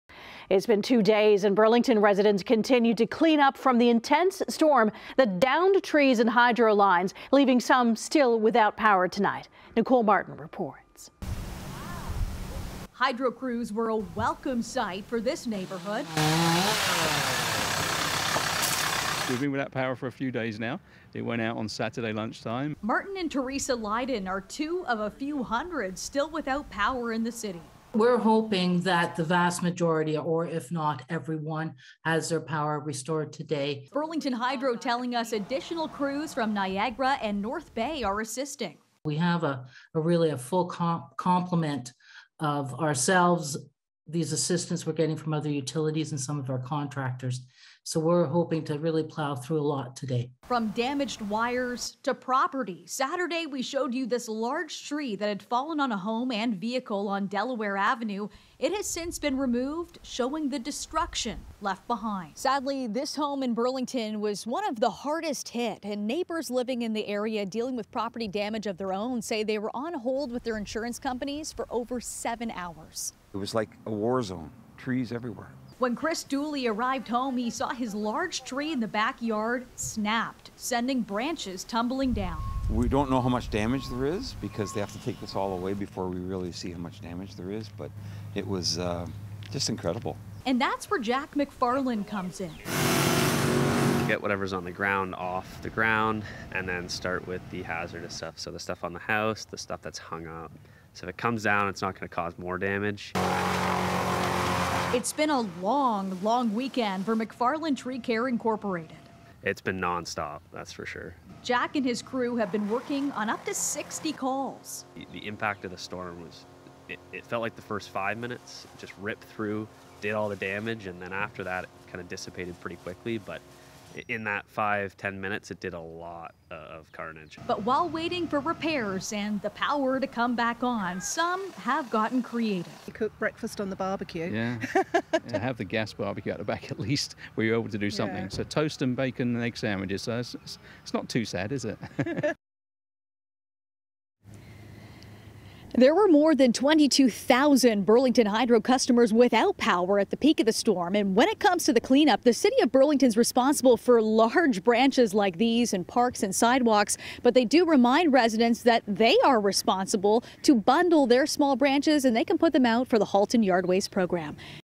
Burlington residents continue to clean-up after the intense storm
Two days after Saturday's intense storm, Burlington residents continue to clean up.

The storm downed trees and hydro lines leaving some still without power today.

Martin and Theresa Lydon are two of a few hundred still without power in Burlington. "We've been without power for a few days now. It went out Saturday around lunchtime," Martin said.

Burlington Hydro tells CHCH News that additional crews from Niagara and North Bay are assisting with the clean-up.

"We are hoping that the vast majority if not everyone has their power restored today," Burlington Hydro said.

Nicole Martin has more of the Burlington storm clean-up.